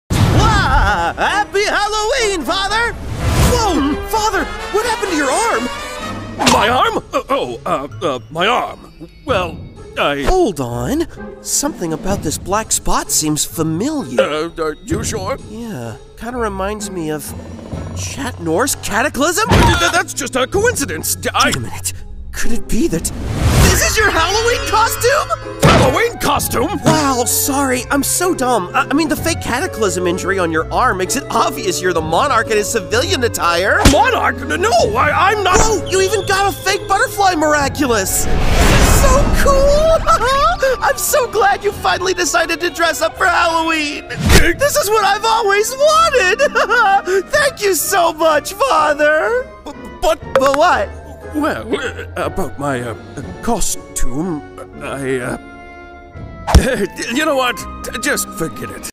Adrien's Happy Halloween
You've got to give Gabriel some points for originality.

FEATURING THE VOICE TALENTS OF:

Me. as Adrien Agreste, Gabriel Agreste